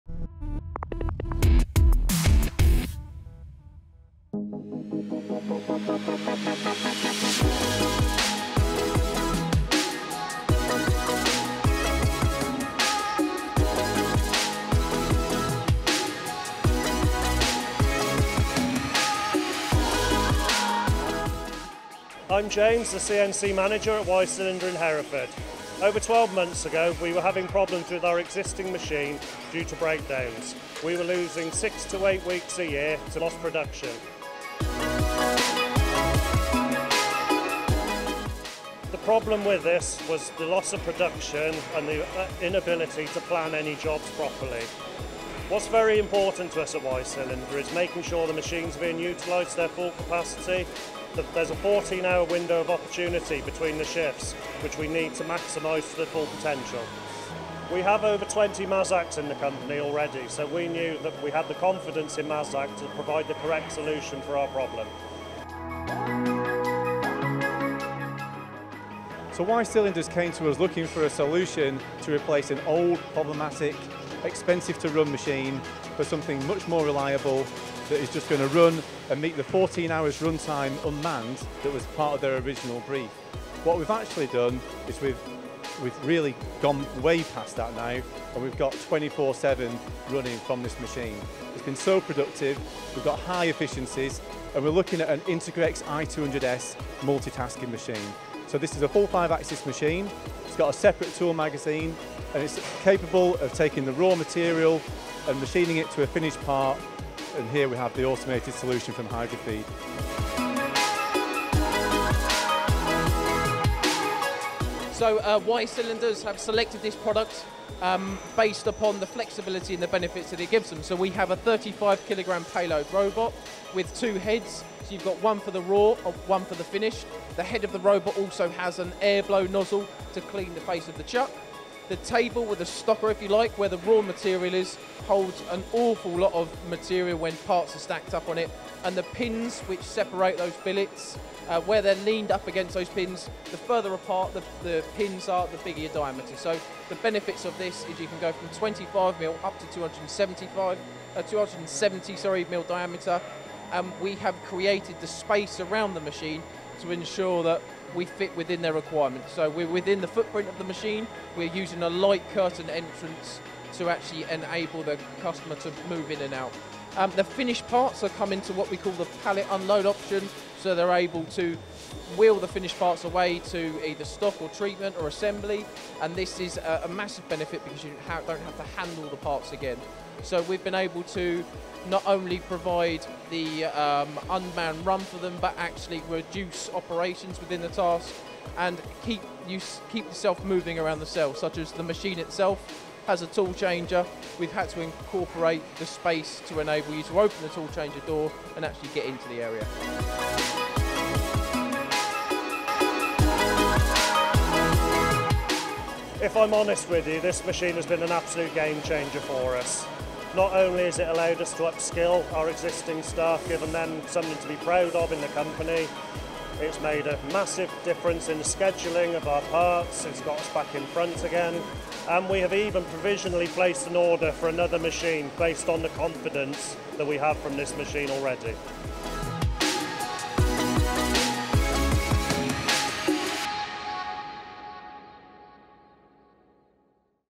A Game Changer At Wye Cylinder With Mazak And Hydrafeed UK
MTD recently visited Wye Cylinder Engineering to discover what challenges they presented to Mazak and Hydrafeed UK and what solutions they offered which have now transformed they way they manufacture with a 24/7 automated and stable process.